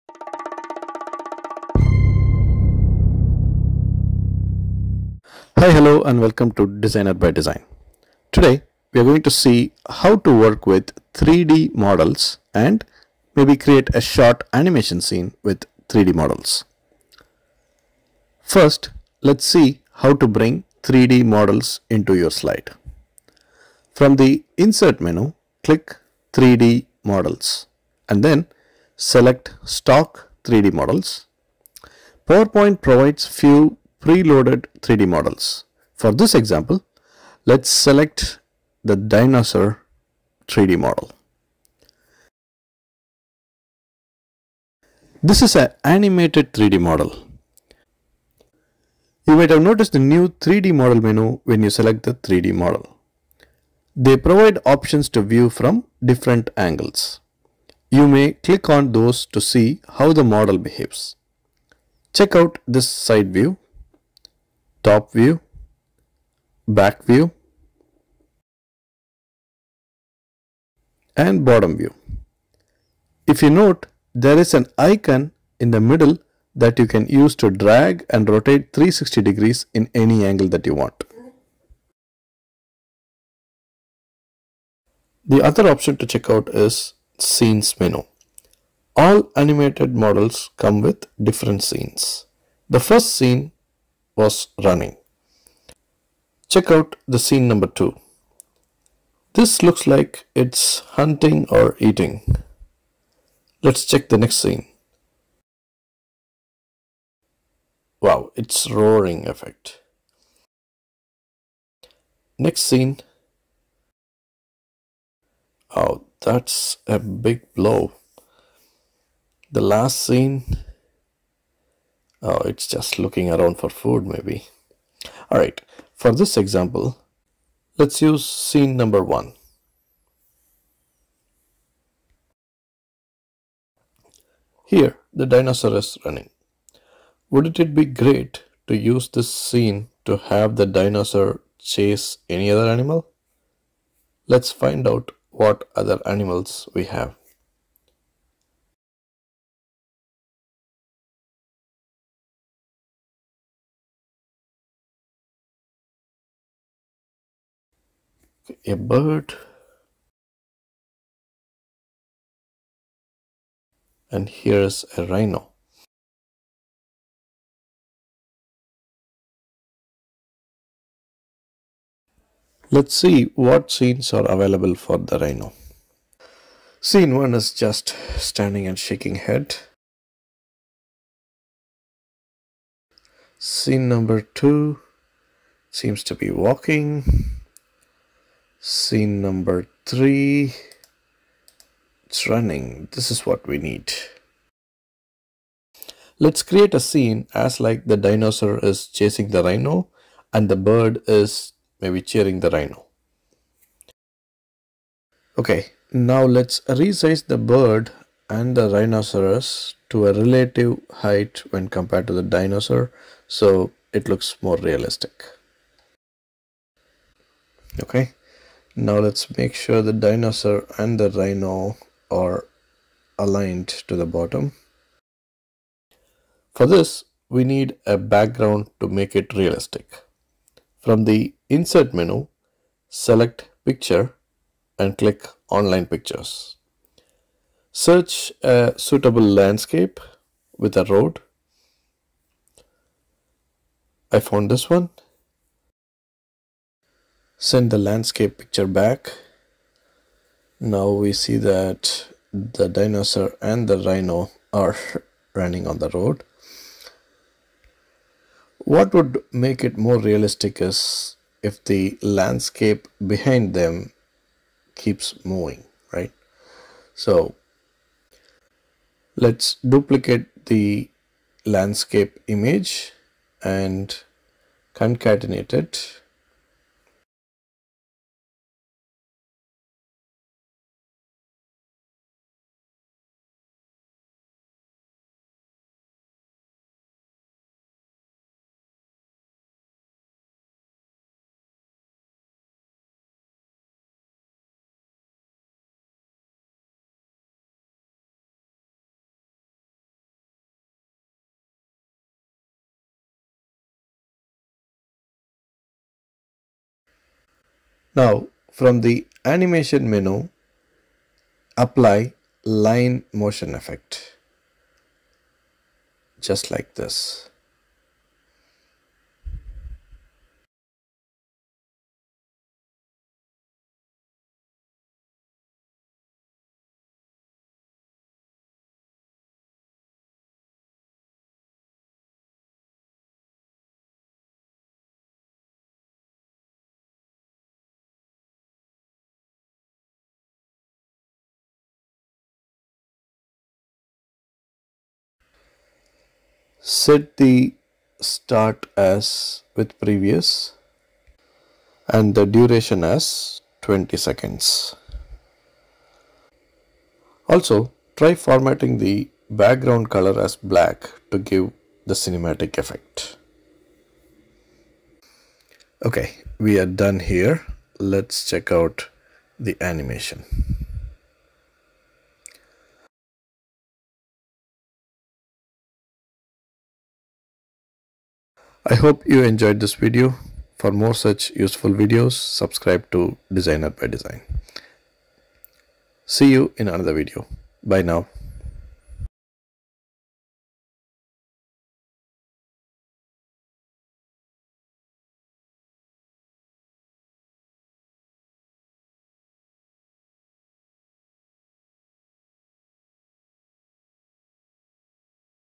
3 Incredible Hacks For 3d Models And Animations In PowerPoint PPT
3 Incredible Hacks For 3d Models And Animations In PowerPoint PPT

00:00 Intro to Dzyner By Design
00:17 Inserting 3D models
00:50 Different views of 3D models
01:14 Rotating 3D models
01:30 Animated 3D model scenes
03:50 Creating Animated scenes with 3D models
06:09 Animated scenes with 3D models
06:20 Subscribe to DzynerByDesign